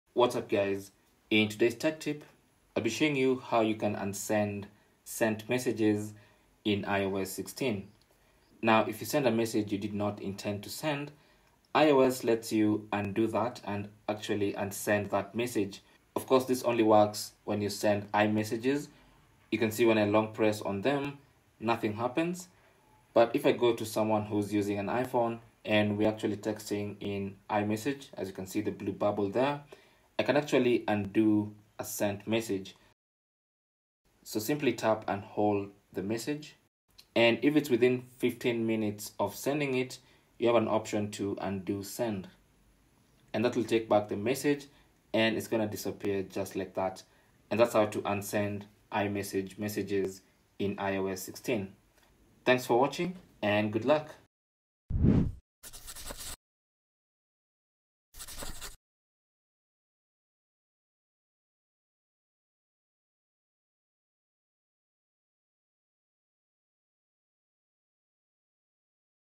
iOS 16: How To Unsend Sent Messages | Delete Sent Message On iPhone
Here's how to unsend sent messages on the iOS 16. Well, maybe you sent the wrong text message to someone without you knowing. With the iOS 16, it is possible to undo the message that you sent.

As long as you make sure you do this within 15 minutes of the sent message. It only works with iMessage.

Timestamps
0:00 - Intro
0:08 - How to undo send on a sent message
0:47 - Outro

Cell Phone Tripod Adapter